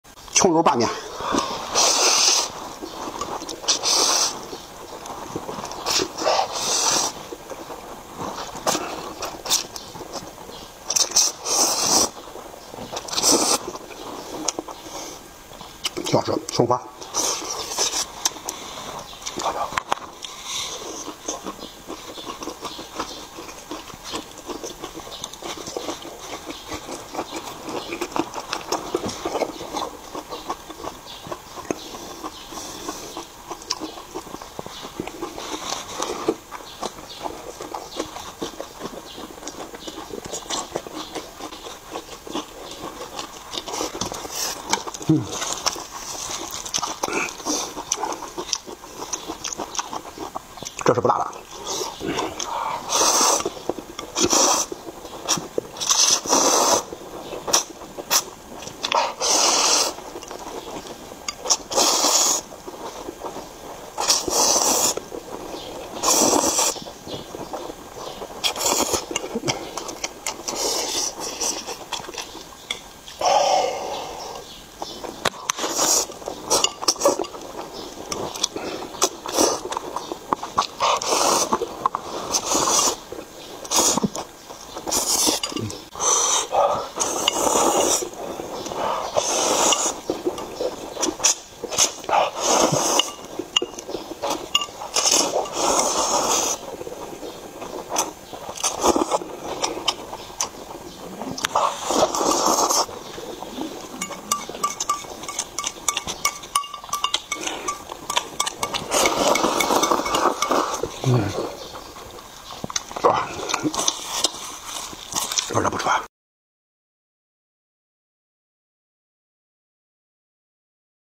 MUKBANG ASMR | EATING FOOD Scallion oil Noodles With Roasted Peppers And Egg
Hello everyone, welcome to Xiaofeng Mukbang Official Channel Share eating food every day